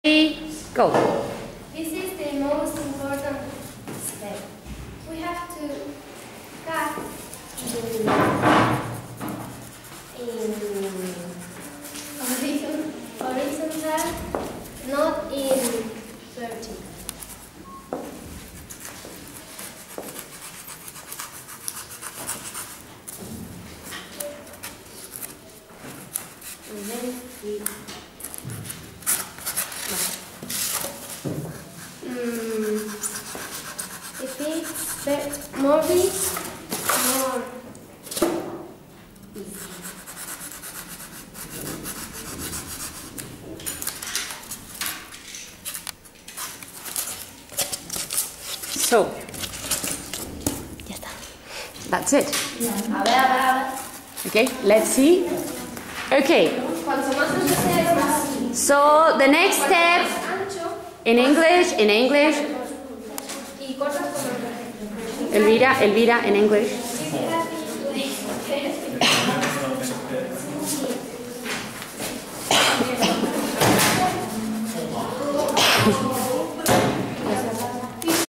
Making Tombstones Workshop (Part 1).mpg
Elvira  shows the rest of the class how to make great looking tombstones with Poliexpand. They will later use them for their Halloween classroom decoration.